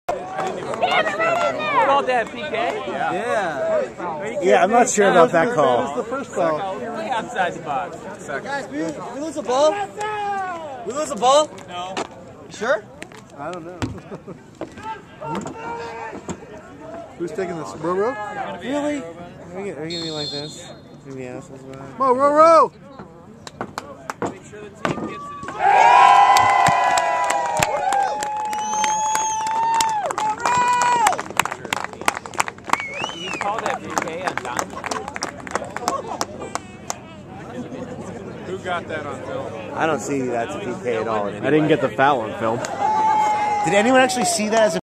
Rodrigo Lopez penalty kick
Portland Timbers @ OSU Beavers (preseason) : March 6 2010 : Corvallis OR

Roro buries the penalty at the death for a 2-0 Timbers win.